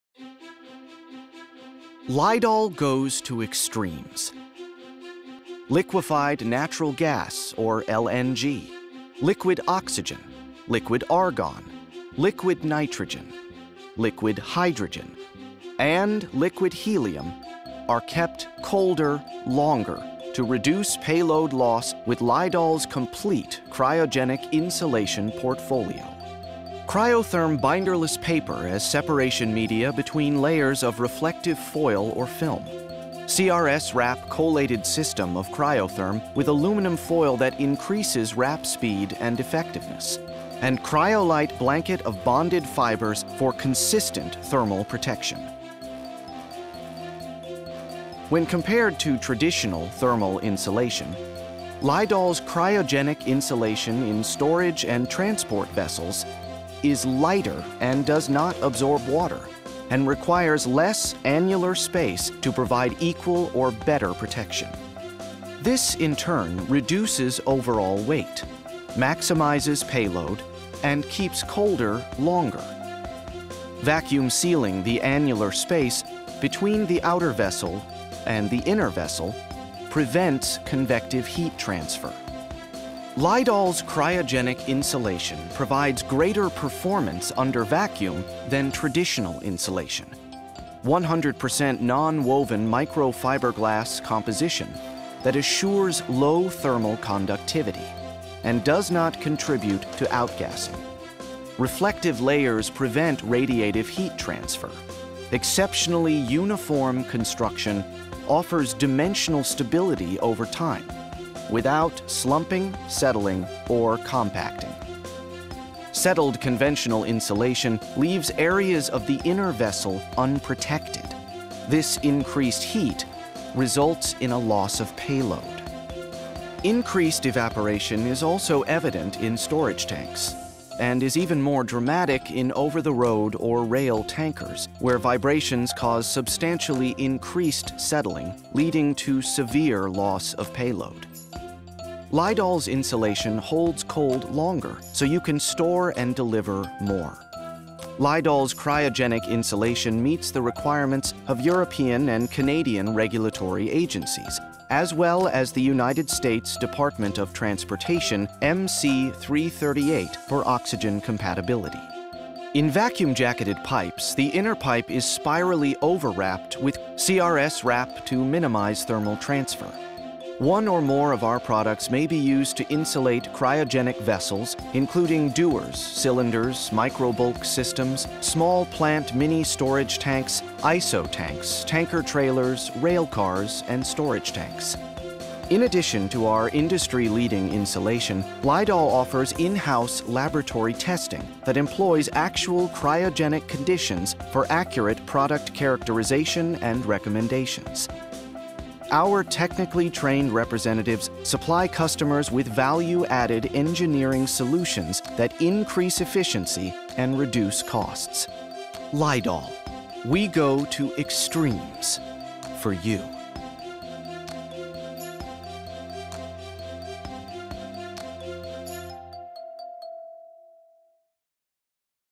Cryogenic Insulation Portfolio from Lydall Specialty Insulation Materials
Introduction to Lydall Specialty Insulation Materials' Cryogenic Insulation Portfolio. See how their Cryogenic Insulation performs when compared to typical insulation. For more information please visit the Lydall Specialty Insulation Materials